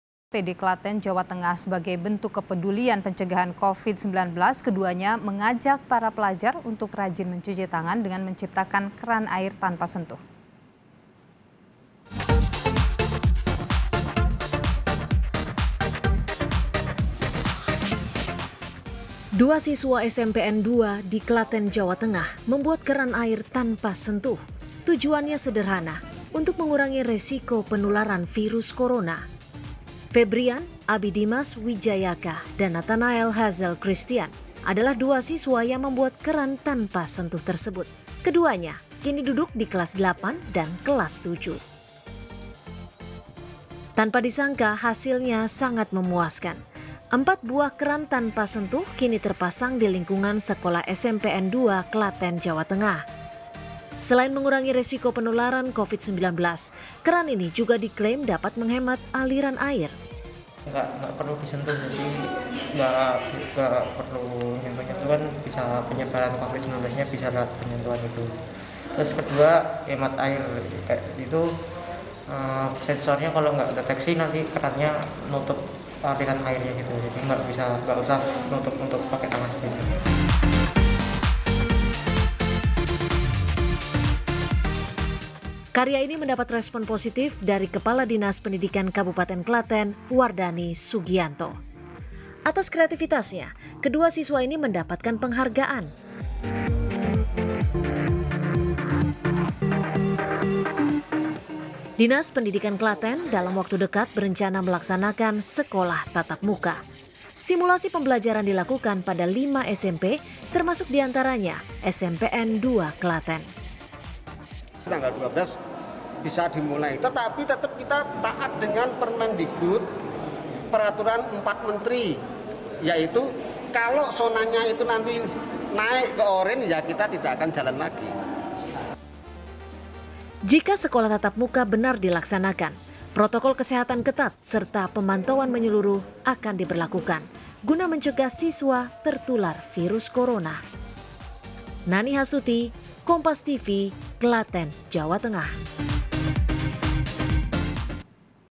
Inovatif! 2 Siswa SMP di Klaten Ciptakan Keran Air Tanpa Sentuh
KOMPAS.TV - Inovasi dilakukan 2 siswa SMP di Klaten, Jawa Tengah.

Sebagai bentuk kepedulian pencegahan Covid-19, keduanya mengajak para pelajar untuk rajin mencuci tangan dengan menciptakan keran air tanpa sentuh.

Dua siswa SMPN 2  di Klaten, Jawa Tengah, membuat keran air tanpa sentuh.

Tujuannya sederhana, untuk mengurangi resiko penularan virus Corona.

Febrian Abidimas Wijayaka dan Nathanael Hazel Christian, adalah dua siswa yang membuat keran tanpa sentuh tersebut.

Keduanya kini duduk di kelas delapan dan  kelas tujuh.

Tanpa disangka, hasilnya sangat memuaskan.

Empat buah kran tanpa sentuh kini terpasang di lingkungan sekolah SMPN 2, Klaten, Jawa Tengah.

Selain mengurangi risiko penularan Covid-19, keran ini juga di klaim dapat menghemat aliran air.

Karya  ini mendapat respons positif dari  kepala dinas pendidikan Kabupaten Klaten, Wardani Sugianto.

Atas kreatifitasnya kedua siswa ini mendapatkan penghargaaan.

Dinas Pendidikan Klaten , dalam waktu dekat berencana melaksanakan sekolah tatap muka.

Simulasi pembelajaran dilakukan pada 5 SMP termasuk diantaranya SMPN 2 Klaten.

Jika sekolah tatap muka benar dilaksanakan, protokol kesehatan ketat, serta pemantauan menyeluruh akan diberlakukan, guna mencegah siswa tertular virus Corona.